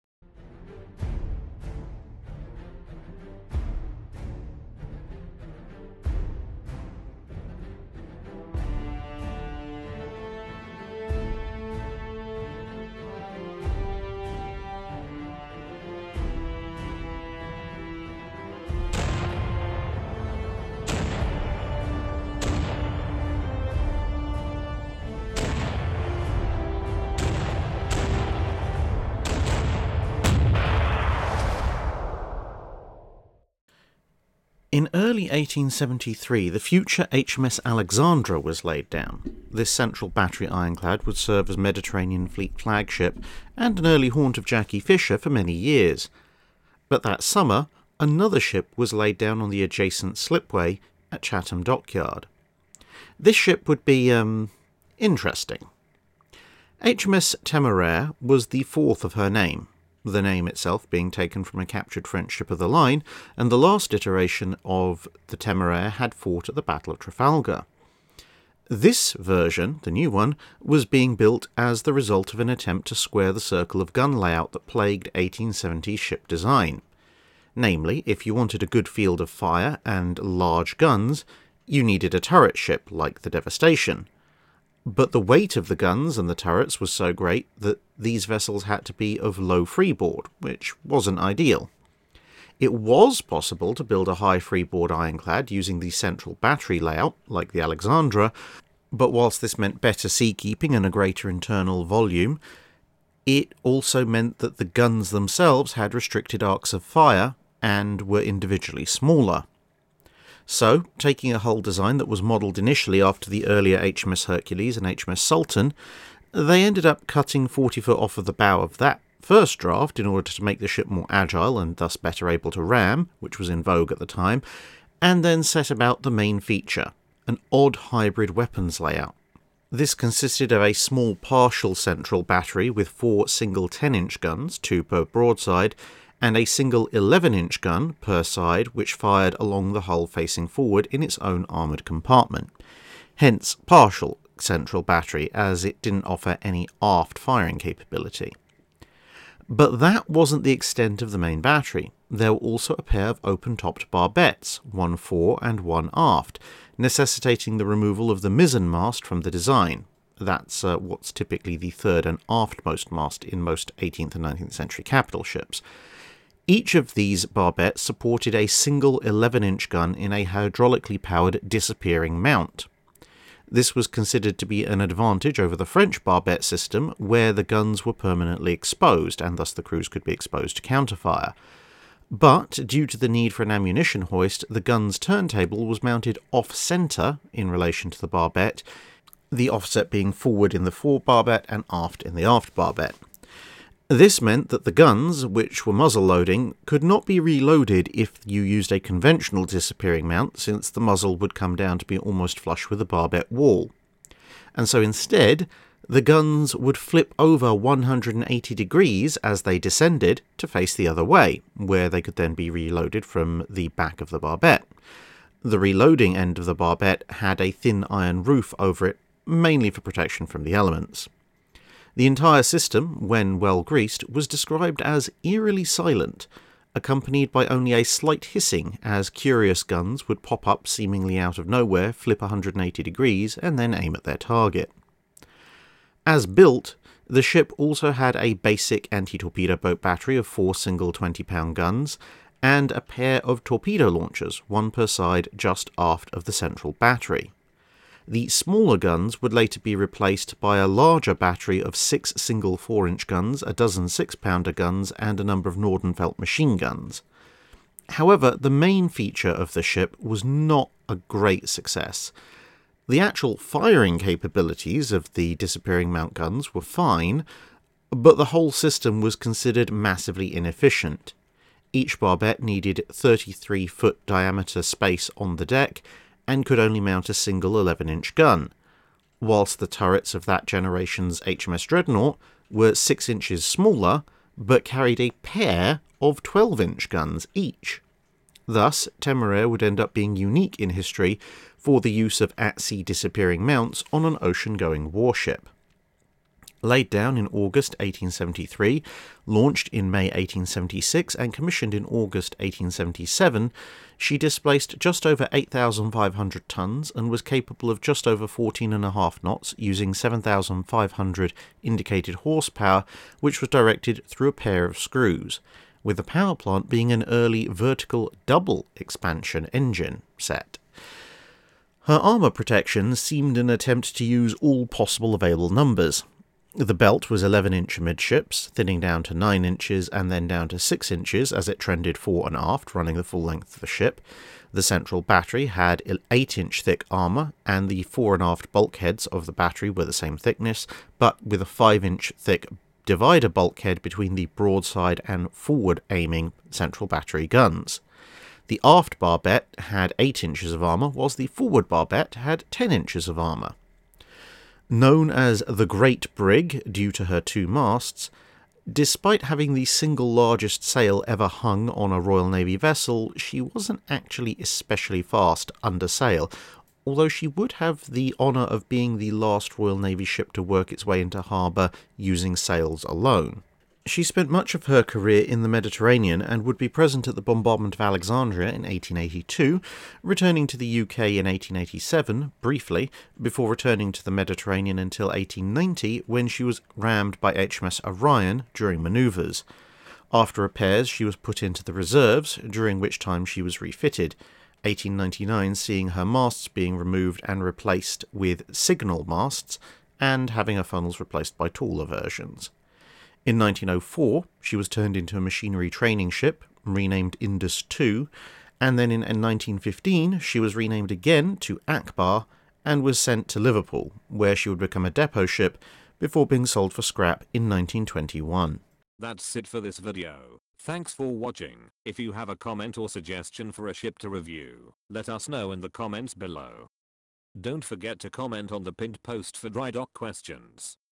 HMS Temeraire (1876) - Guide 275
HMS Temeraire, a unique ironclad of the Royal Navy, is today's subject.